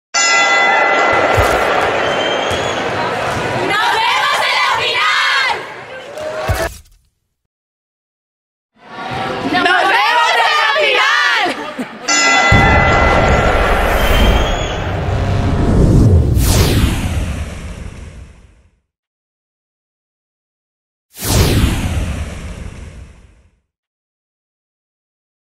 Previa Final Copa Colegial femenina. Estudio vs Los Sauces Torrelodones
La Gran Final de la Copa Colegial Femenina ya está aquí. Estudio y Los Sauces Torrelodones rivalizarán este viernes 18 de mayo (17:00 horas) por el cetro de la mejor competición escolar de Europa en Madrid.

Muchas gracias.

Baloncesto para aprender y disfrutar desde 1999

Tu tienda de baloncesto